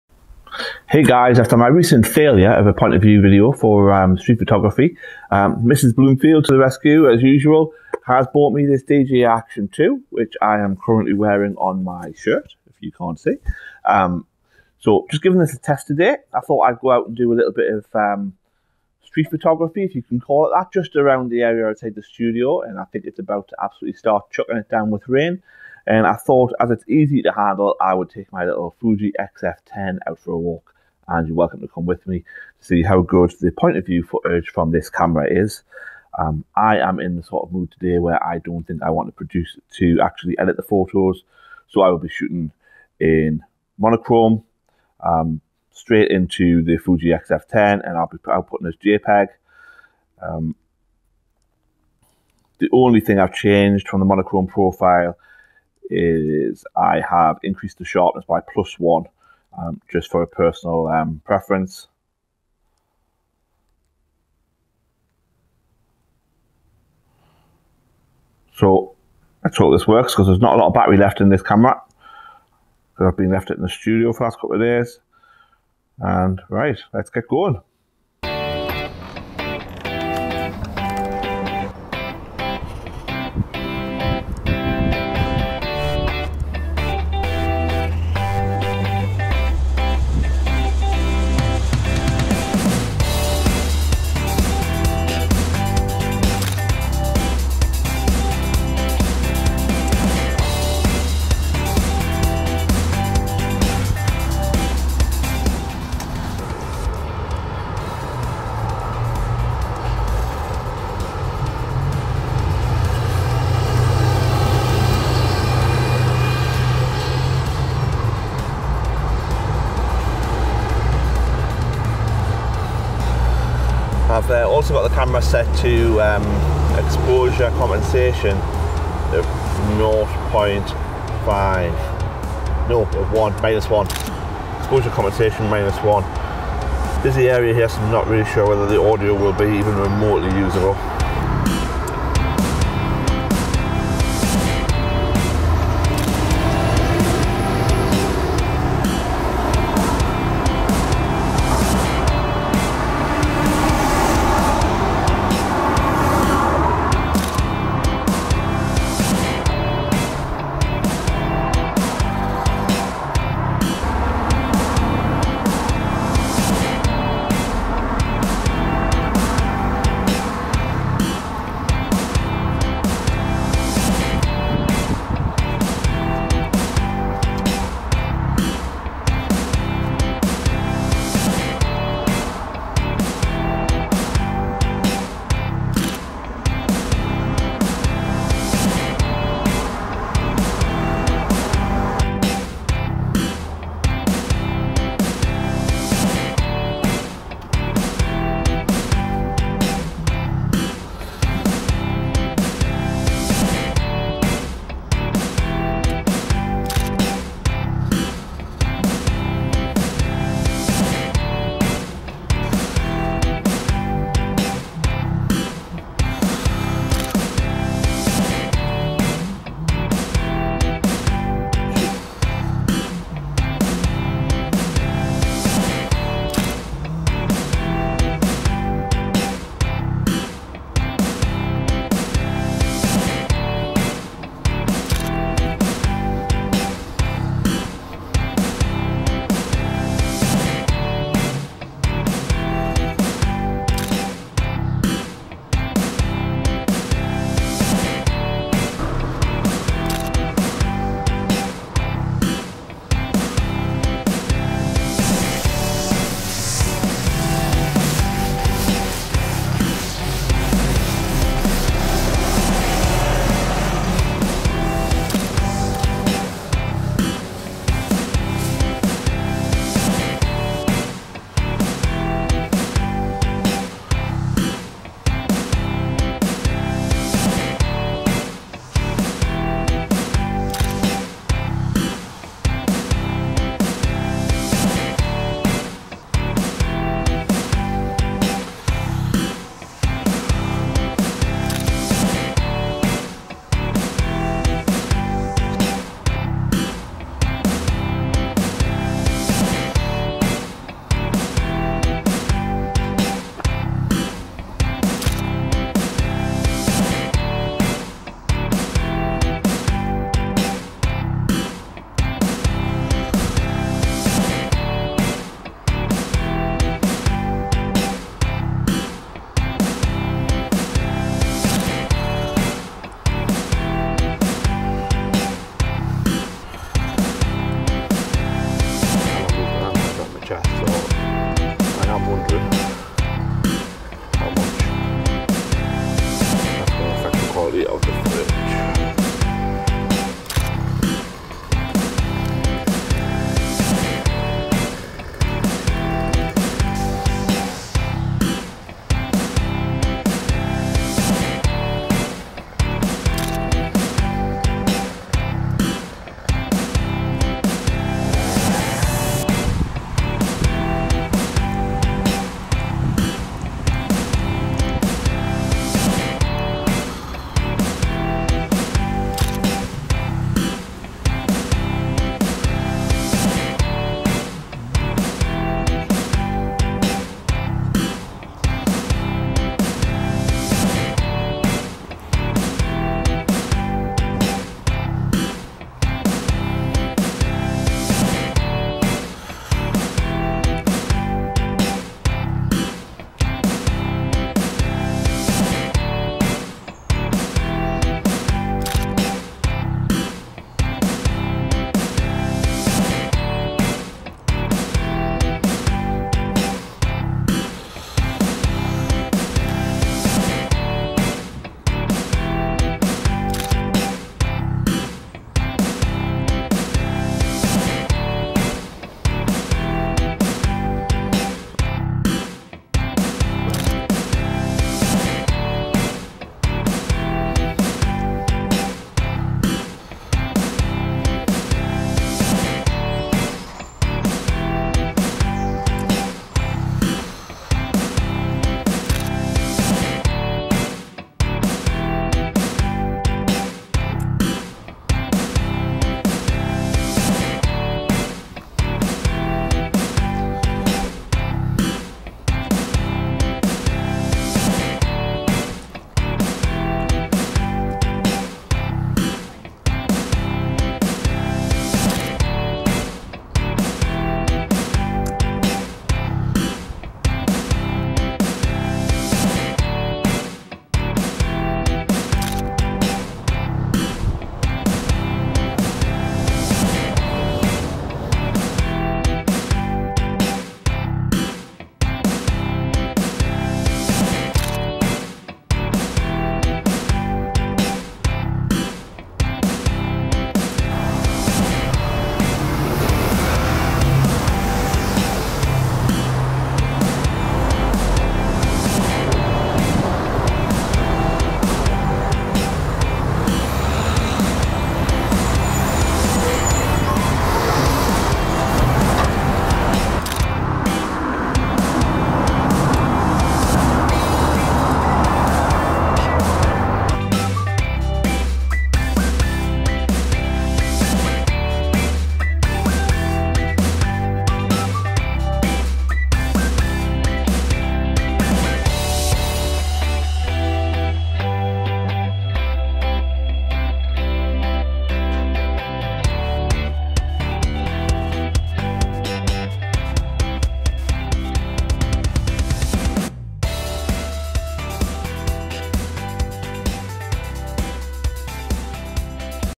POV Street Fuji XF10 testing the DJI Action2, in Houghton le Spring
POV Street on Fuji XF10 testing the DJI Action2, around the streets of Houghton le Spring

Testing out my DJI action 2 as a point of view camera for capturing street photography with my Fuji XF10 shooting straight to monochrome JPG.

B16 Houghton Business Centre, Lake Road, Houghton le Spring. DH5 8BJ